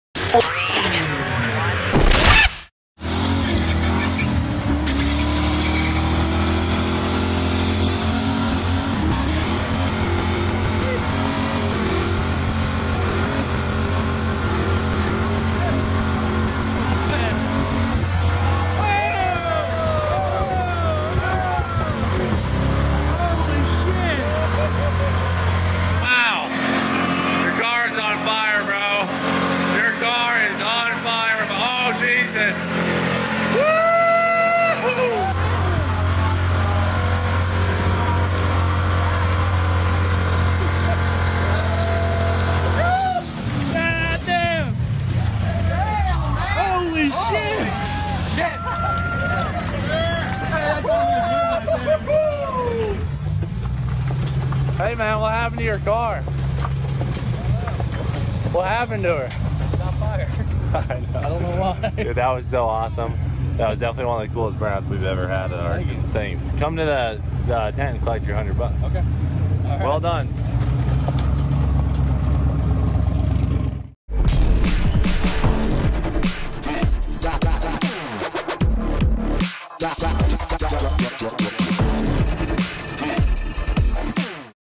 Car Catches on FIRE During BURNOUT!
Holy sh*t this was INSANE! The Street Car Takeover burnout contest winner was an absolute MAD MAN! Not only did he do a big nasty burnout, his tires also caught on fire and he just keeps going! Wtf!?